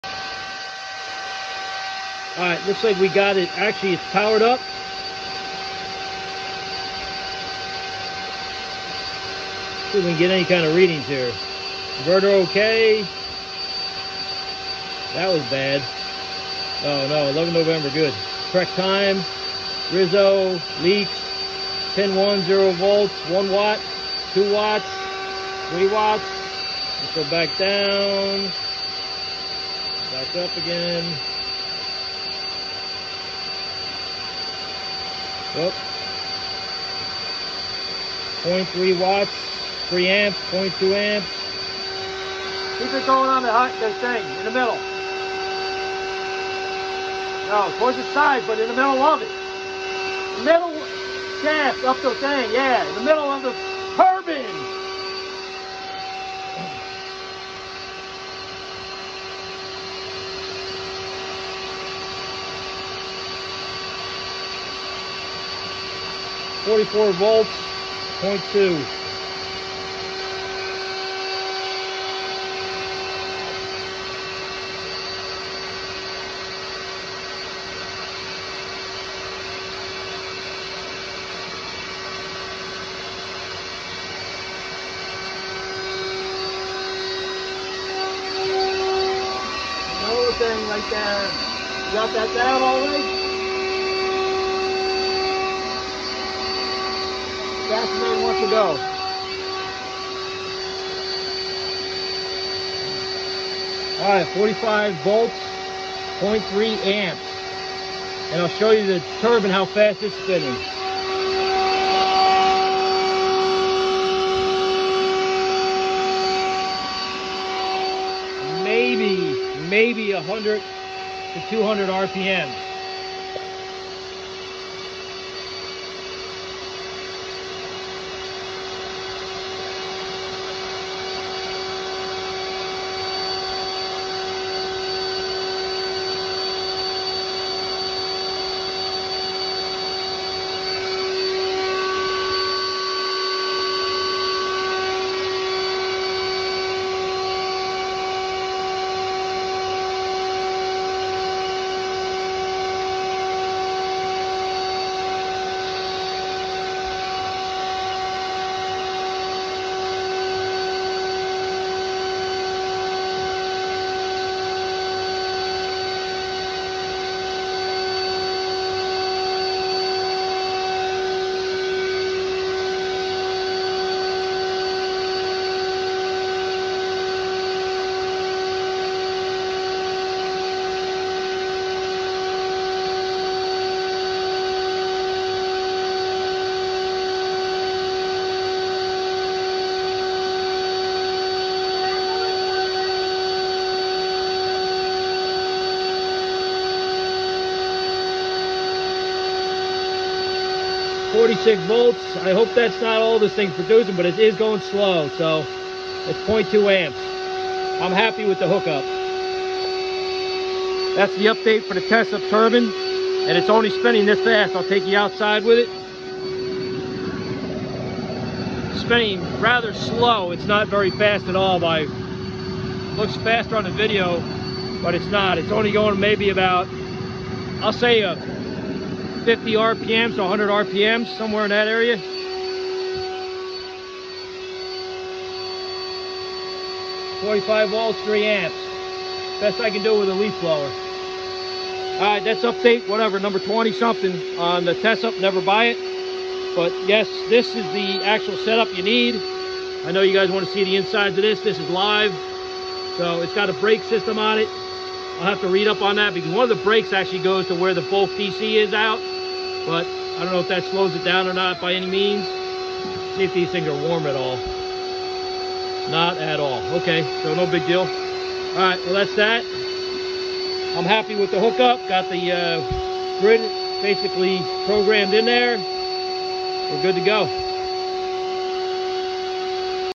Vertical Wind Turbine Part 25 Victory, Never buy Tesup. Powering up the inverter￼.
This video covers the speed of the wind turbine with a leaf blower, maybe 100 RPM, low voltage (50 Volts) but enough to produce power to the grid. It goes through the Aurora interface box to the 6000 Watt Aurora inverter. The Aurora inverter had to be programmed with a power curve for the wind.
Using the Aurora installer CD & a CD external drive off my laptop, and a dongle cord from "US conversions", Plus a 60 V DC power supply to turn the inverter on. The inverter came on at 50 Volts and drops off-line around 25 Volts. One problem setting the seconds that it stays on in between wind, is anywhere up to 9000 seconds which is extremely long,  the preset was 600 seconds which is 10 minutes, I cut it back down to 300 seconds which is five minutes, even that is a long time for inverter just to stay on. As Time goes on I may even lower it down to two minutes. I am no electrical engineer, but while it’s staying on I believe it is actually using the AC power from your house to stay powered. Can anyone out there tell me if that is true or not?  . Shout out to Harmony turbines in Pennsylvania,US.  They are getting their Prototypes tested and hopefully by next year sometime they will be in production with a Turbine and Generator that is not too big￼￼.